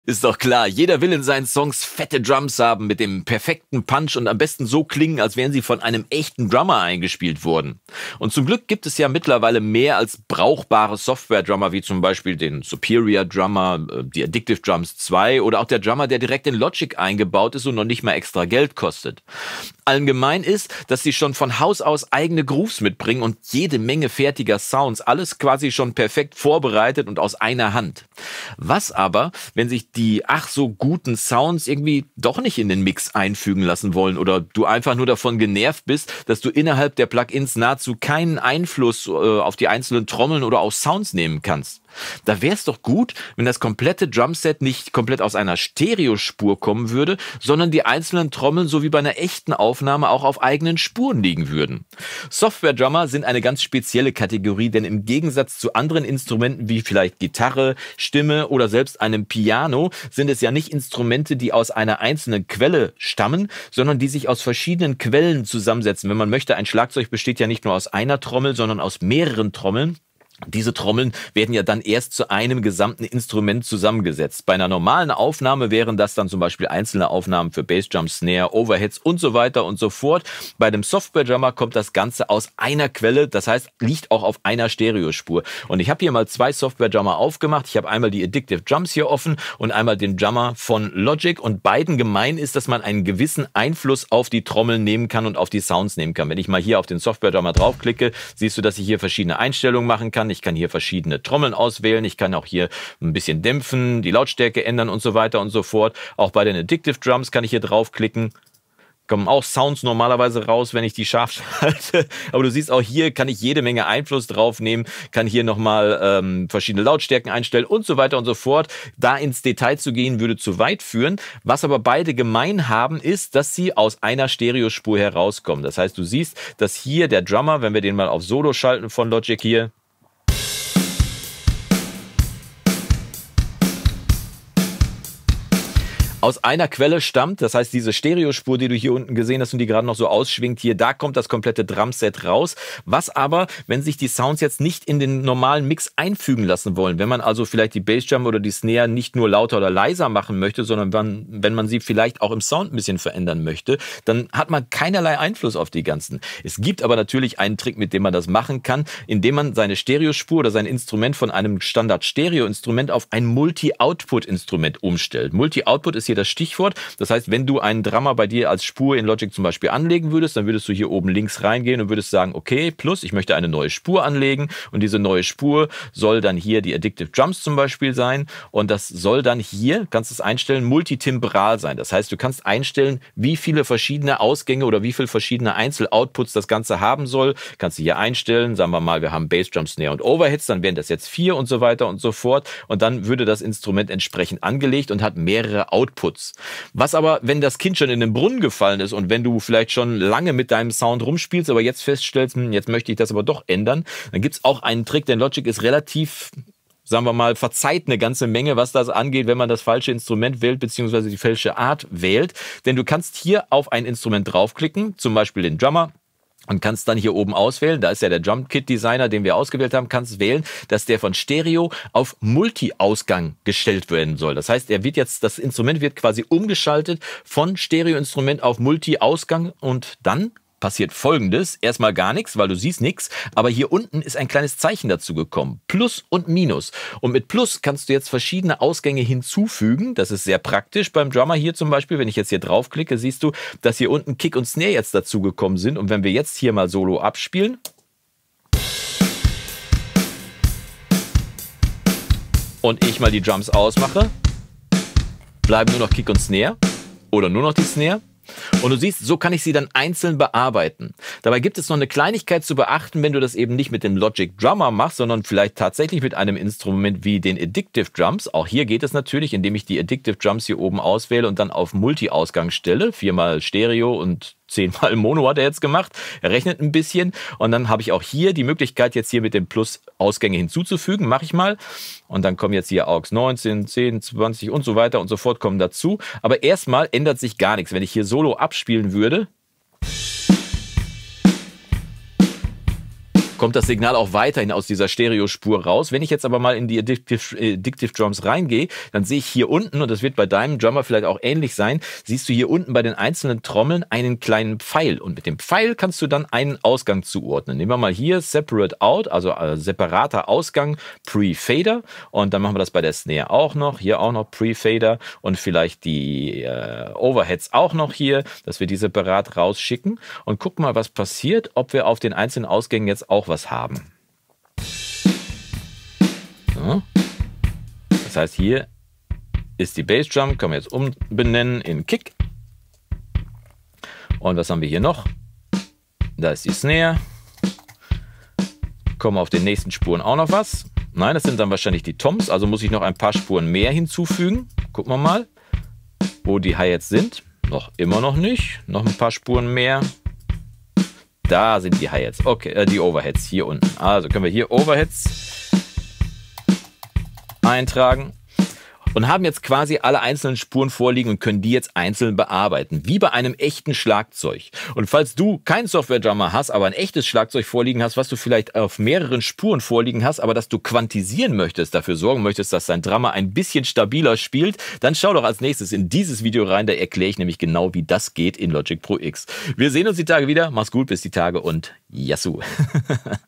Einzelne Outputs für Addictive Drums, Superior Drummer & Co | Tutorial | Recording-Blog
✅ Besser als jedes VST-PlugIn, günstiger 💰 als Du denkst!

Wenn Software-Drummer, wie Addictive Drums 2, Superior Drummer, Logic Drummer usw. keine einzelnen Ausgänge in Logic anbietet, dann gibt es einen einfachen Trick, das zu ändern. Und das funktioniert auch für Kontakt 5 und andere!